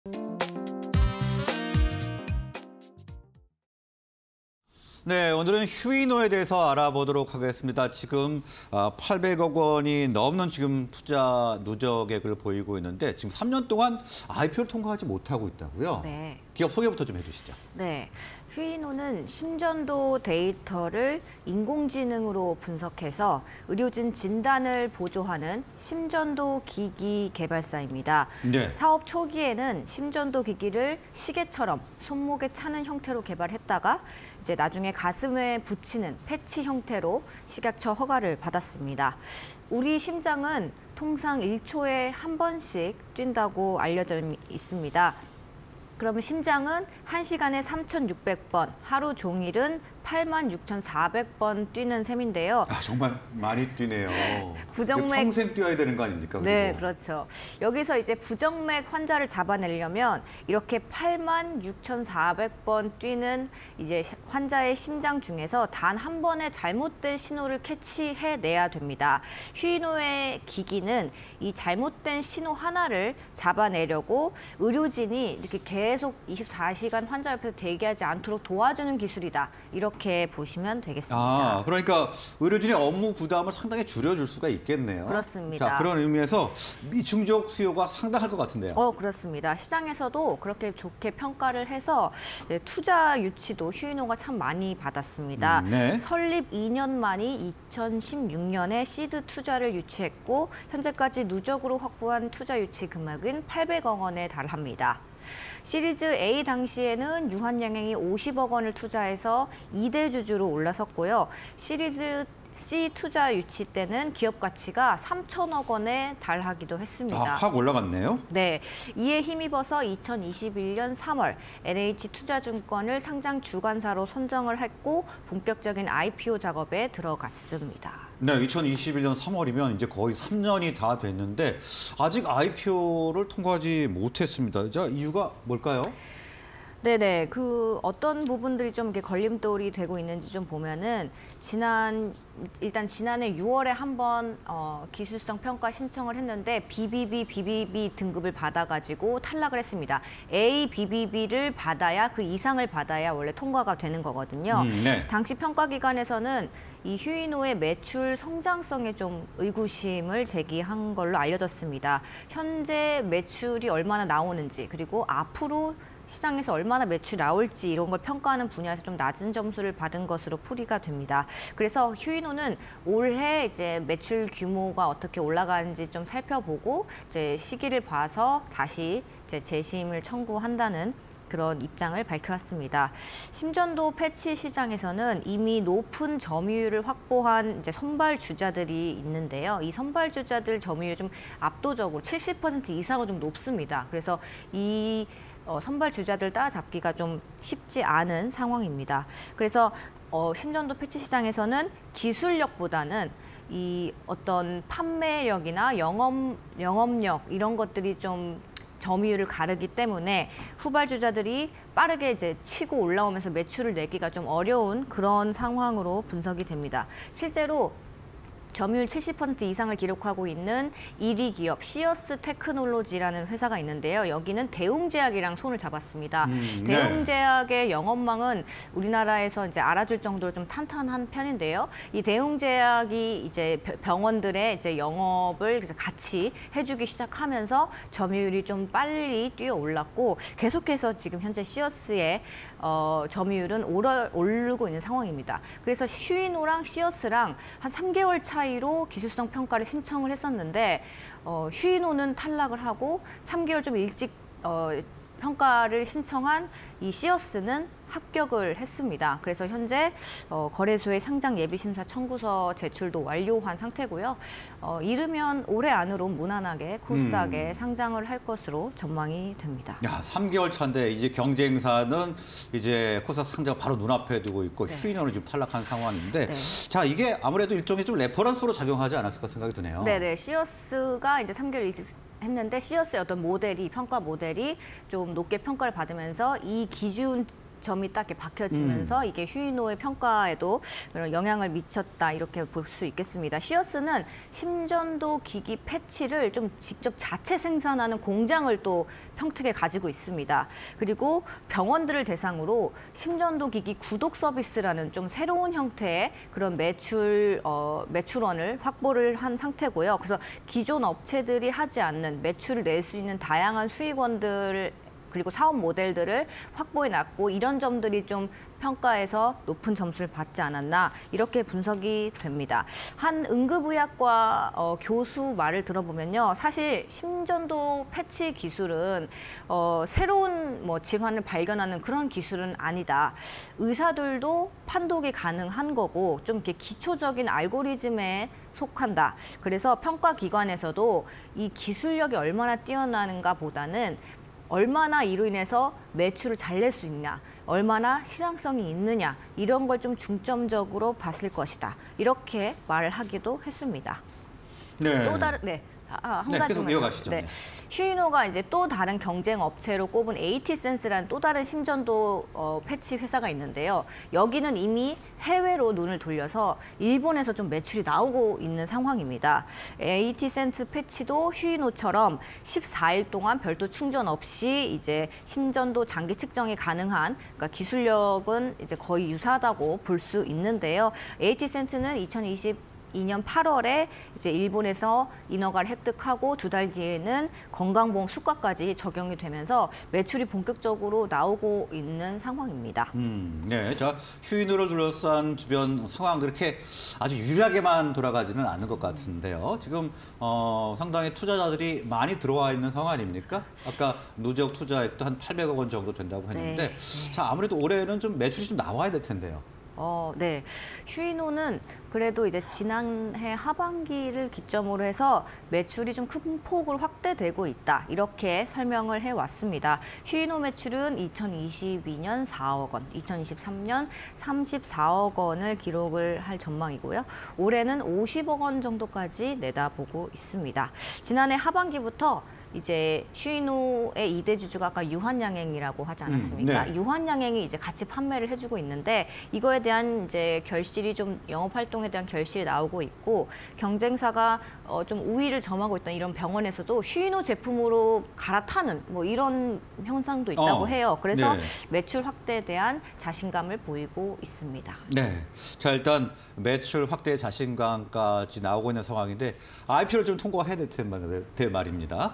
3년째 IPO 안갯속... 휴이노, 기평 문턱 못 넘는 이유는_기자들의 백브리핑 (20240124)
Q. 주요 사업은?
Q. 기관투자자 러브콜 지속... IPO 착수?
Q. 3년째 IPO 안갯속... 이유는?
Q. 3년째 IPO 공회전... 걸림돌은?
Q. 심전도 패치 시장... 기술성보단 시장성 중요
Q. 올해 성장 '청신호' 기대해볼까?